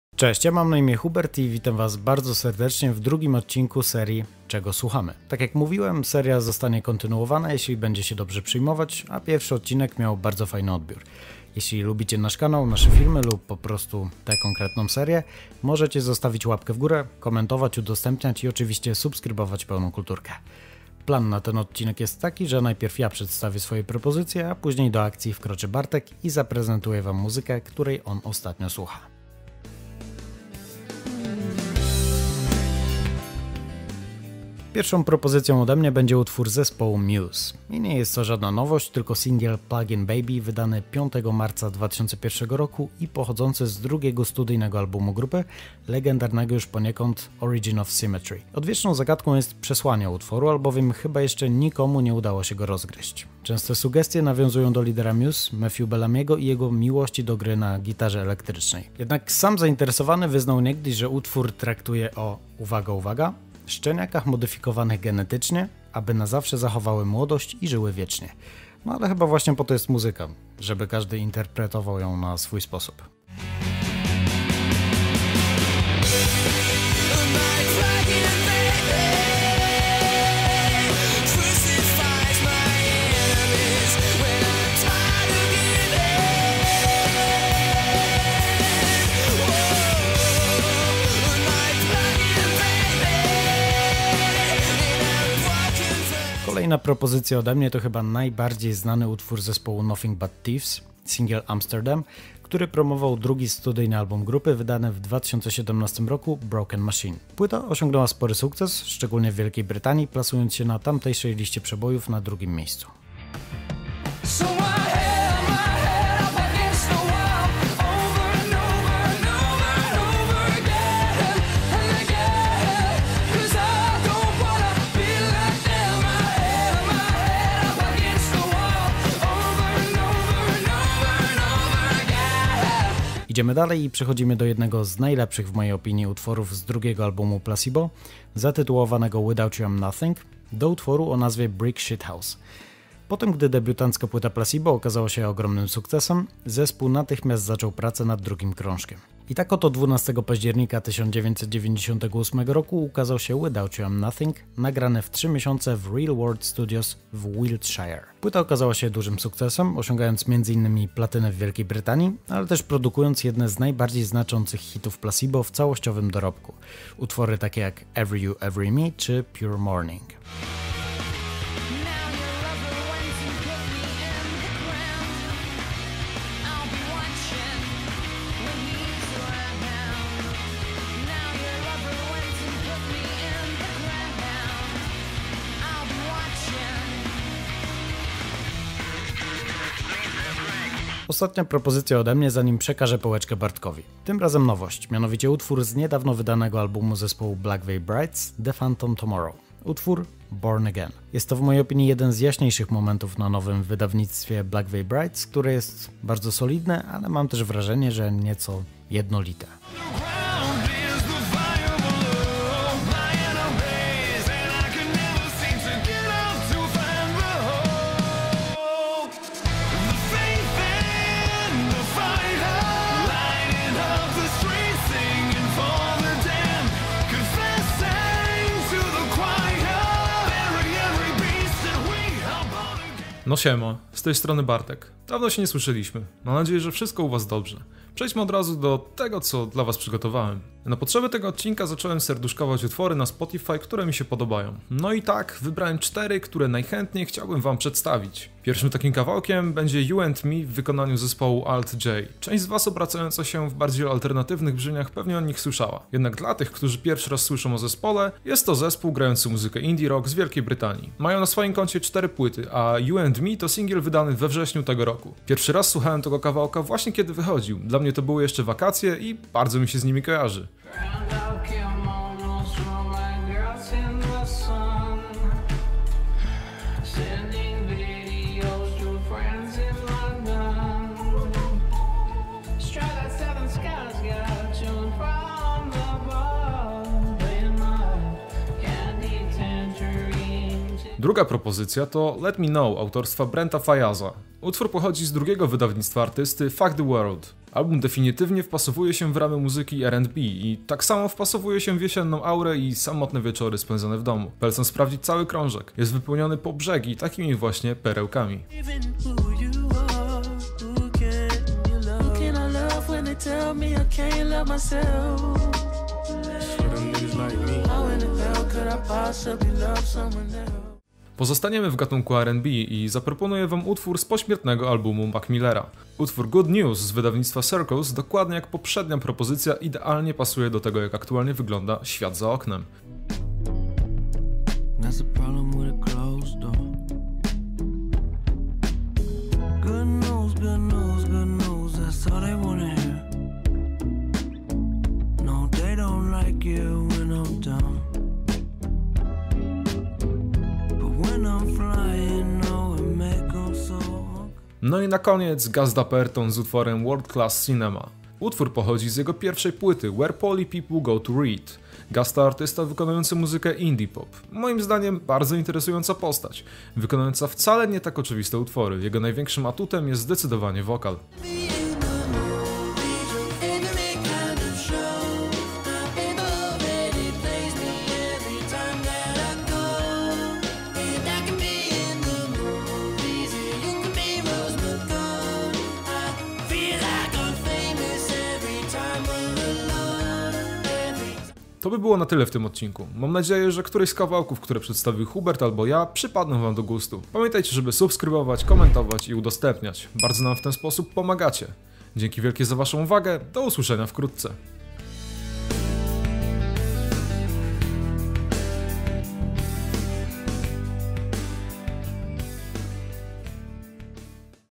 CZEGO SŁUCHAMY W LISTOPADZIE? (MUSE, MAC MILLER, PLACEBO I INNI)
Czego słuchamy w listopadzie? Słuchamy Muse, Mac Millera, Placebo i wielu innych wykonawców. W tej serii po prostu polecamy Wam dobrą muzykę.